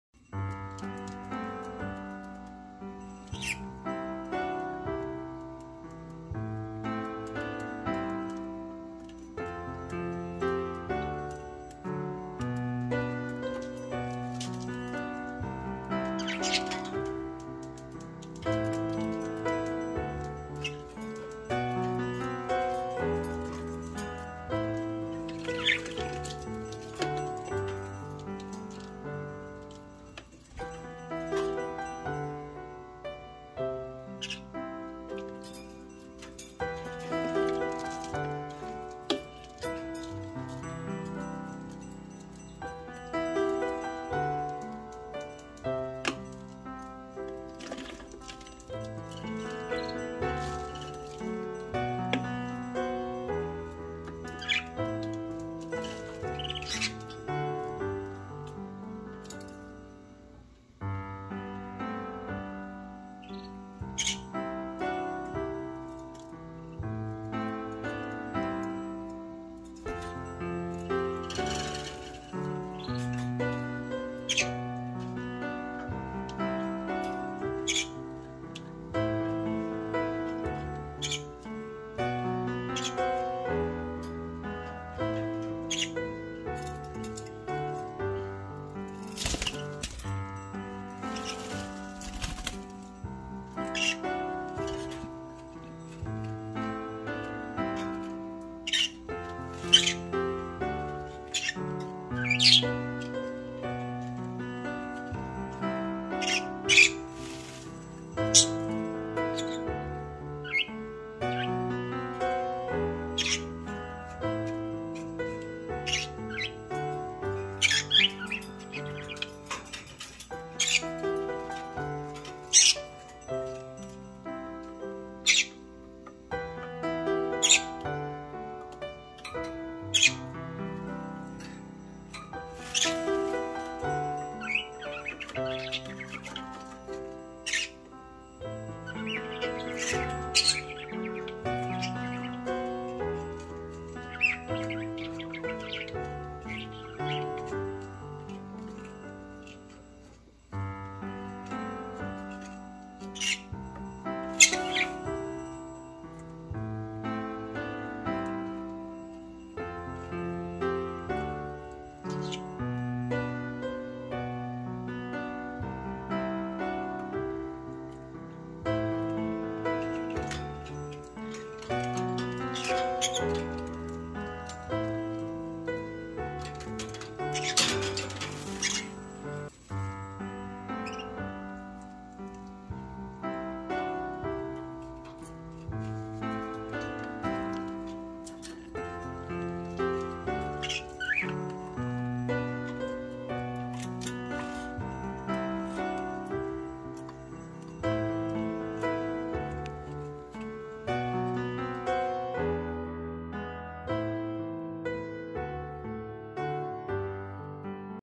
10 weeks old @conure learned how to swim
Trying to get along with the  10 weeks old conure @aprilponte1130